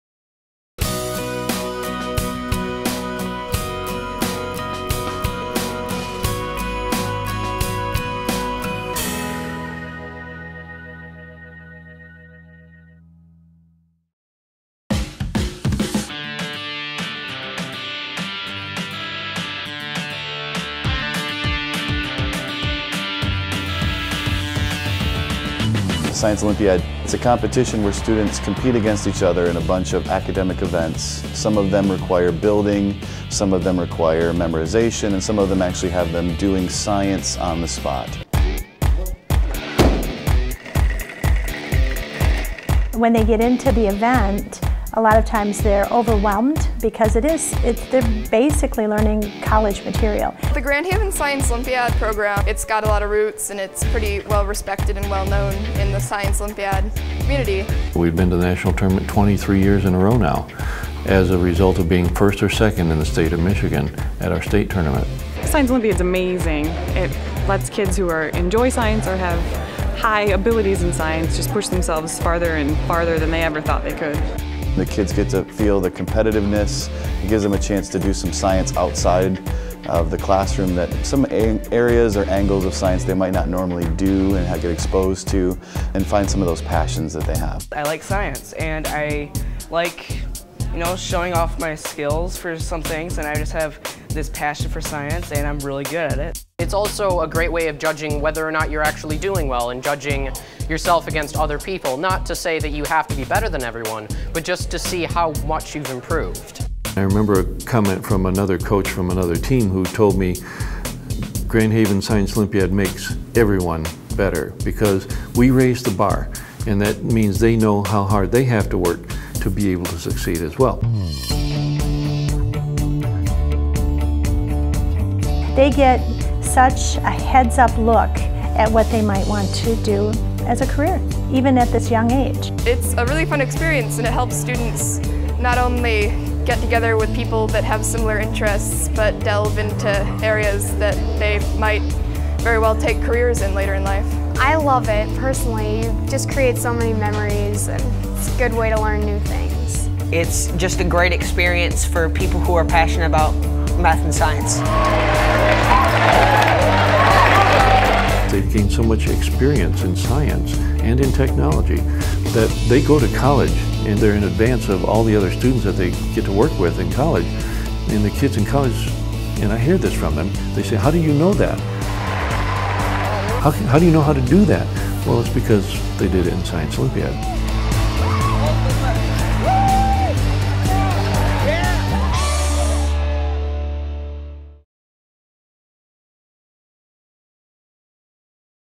Experimenting More - Science Olympiad at Grand Haven Public Schools
​The Science Olympiad team for Grand Haven Public Schools has competed at nationals 23 years in a row! The program is an excellent outlet for students who are passionate about science and math, allowing them to take that interest beyond the classroom.